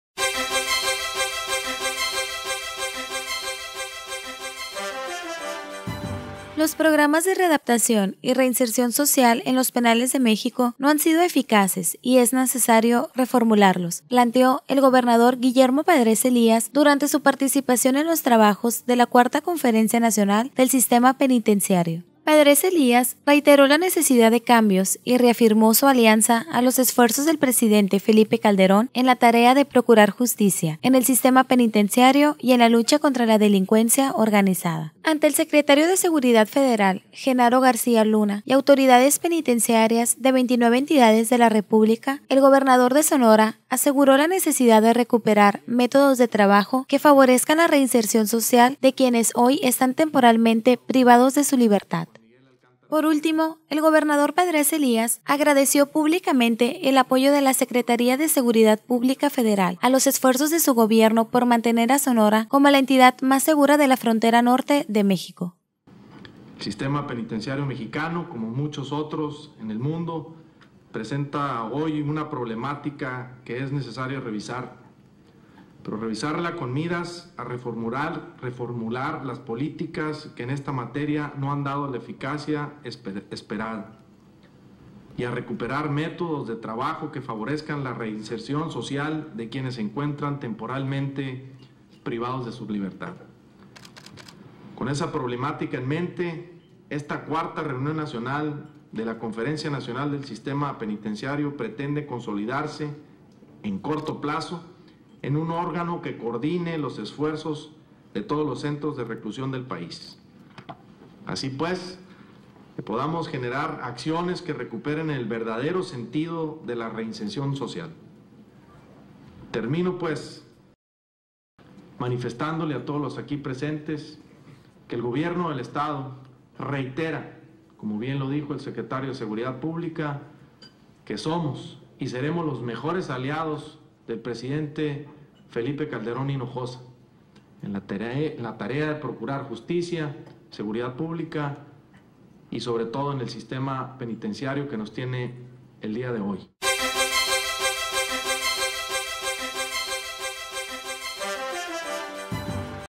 Guillermo Padrés participó en IV Conferencia Nacional del Sistema Penitenciario. 12-11-2010
San Carlos, Guaymas, Sonora.- Los programas de readaptación y reinserción social en los penales de México no han sido eficaces y es necesario  reformularlos  planteó el Gobernador Guillermo Padrés Elías durante su participación en los trabajos de la IV Conferencia  Nacional del Sistema Penitenciario que finalizó hoy en este destino de playa.

Fecha del evento: 12 de Noviembre del 2010